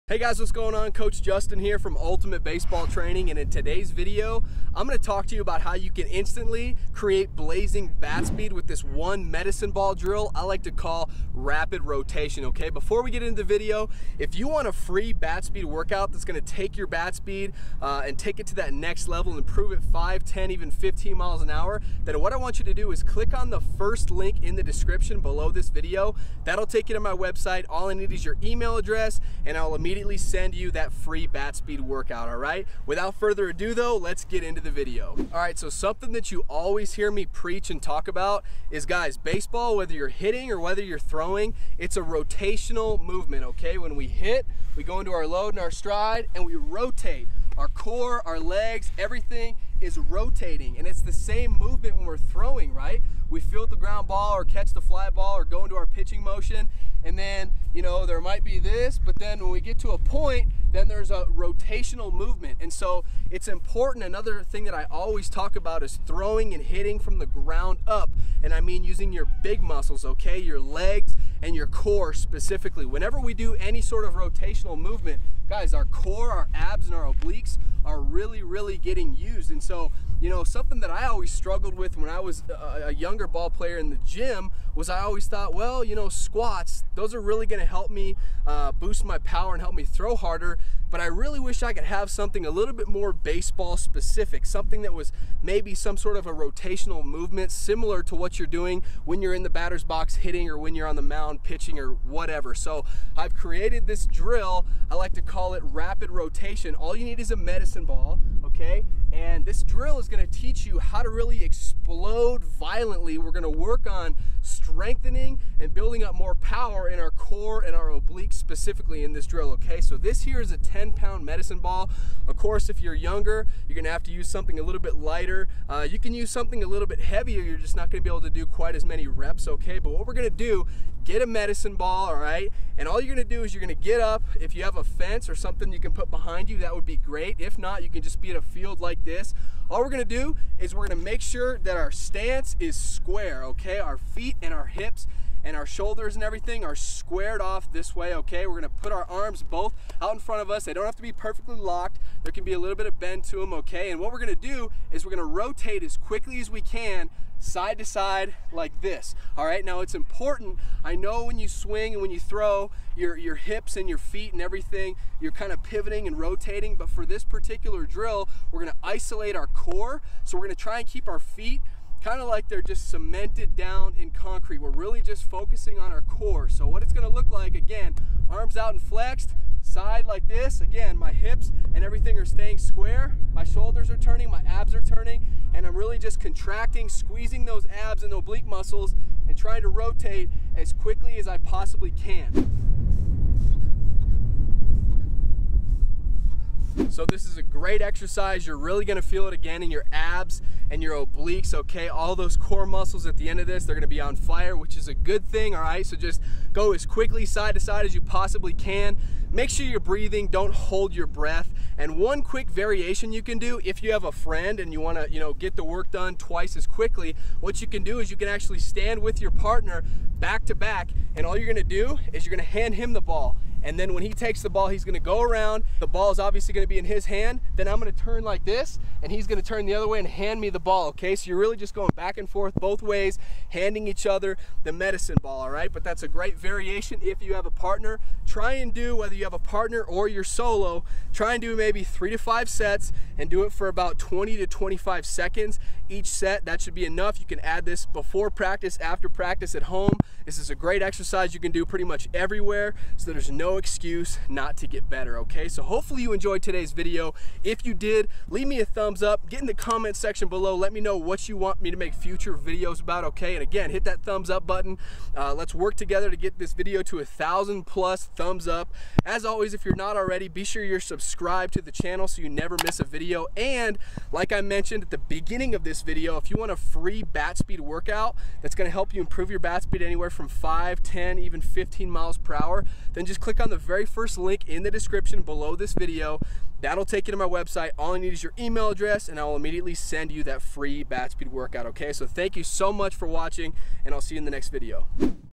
INSTANTLY Create "Blazing" Bat Speed (RAPID ROTATION DRILL!)
INSTANTLY Create "Blazing" Bat Speed (RAPID ROTATION DRILL!) In today's video, Coach Justin will show you how to instantly improve your bat speed with one medicine ball drill!